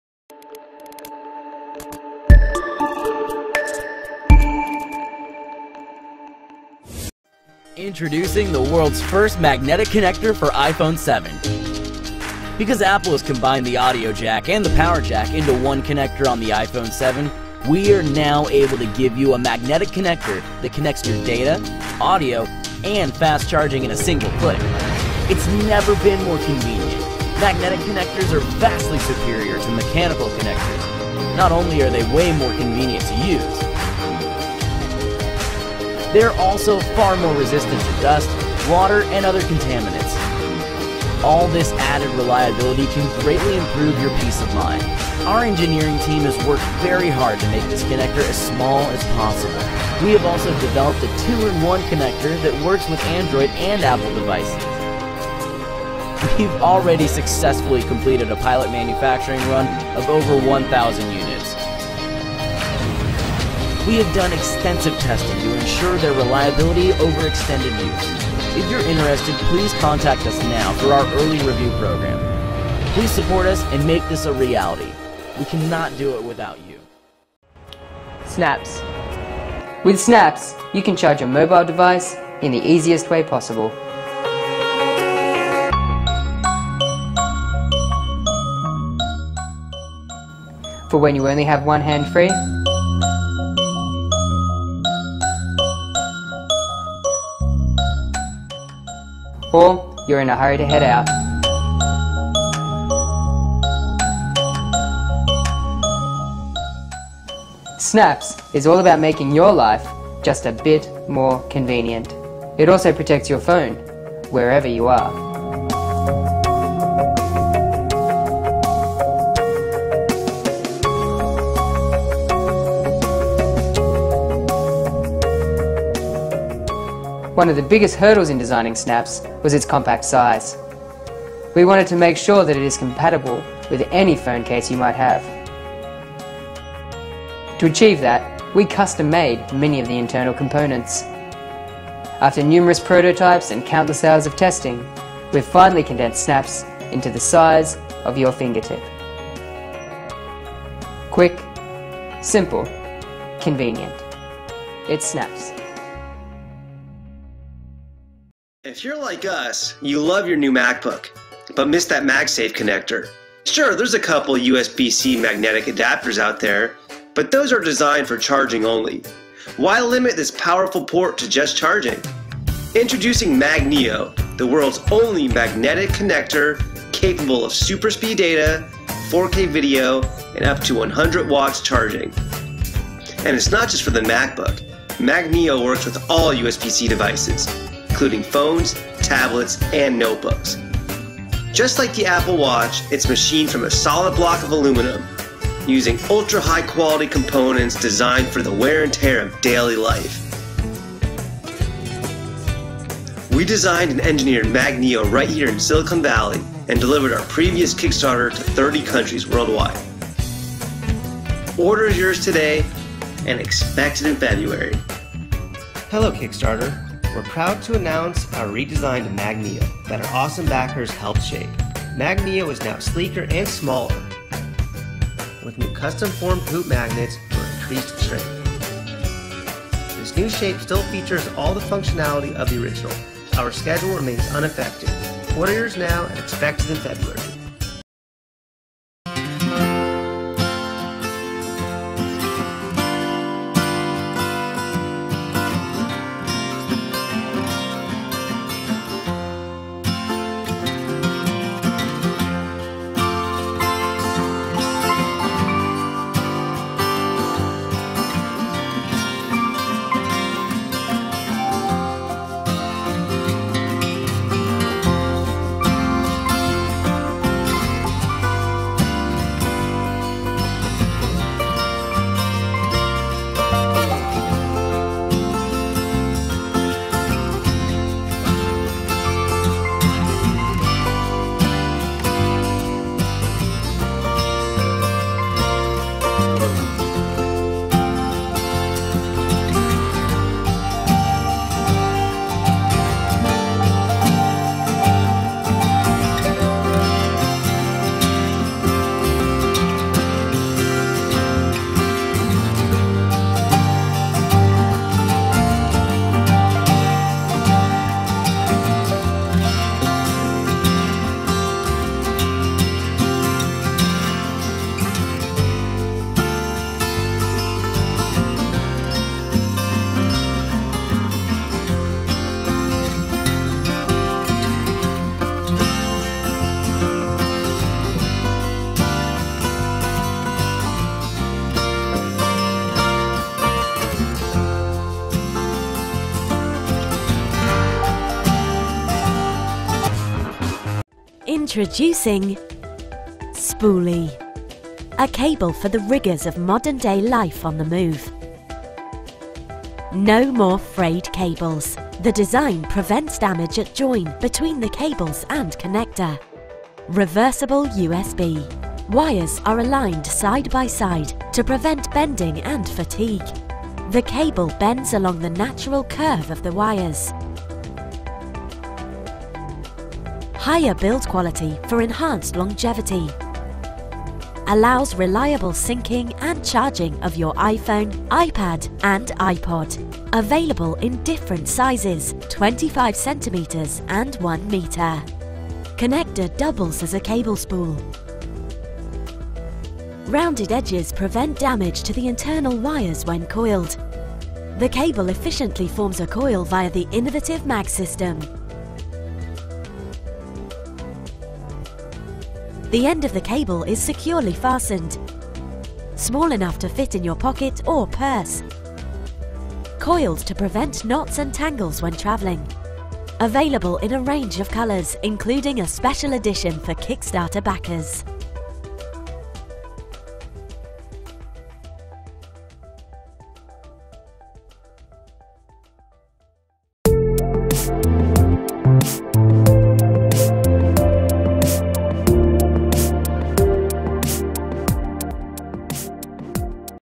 5 Best Magnetic USB Cable / Magnetic Charging Cable for Your Android and Iphone & Laptop#02
5 Best Magnetic USB Cable / Magnetic Charging Cable for Your Smartphone - Android and Iphone & Laptop #02

1. MagBolt: World's 1st Magnetic Connector for iPhone 7&Android
2-in-1 Magnetic connector for iPhone & Android devices. The first magnetic connector to work with iPhones and Android.

2. ZNAPS -

ZNAPS is a magnetic adapter specially designed to  seamlessly fit your mobile devices and cables, so you can charge and sync with the peace of mind that your devices are safe and protected.

3. MagNeo: First True USB-C Magnetic Adapter
The Only MagSafe style USB-C Adapter with Charging + Data + Video. For all USB-C Notebooks, Tablets and Smartphones + 2016 Macbook Pro.

4. X-Connect: World's 1st Cross-device Magnetic Cable
World's first cross-device compatible magnetic cable, means you only need 1 cable for all phones.

 5. Spooly - World's First Magnetic Charging Cables
Incredibly-portable and highly-durable charging cables featuring the first ever Mag-System.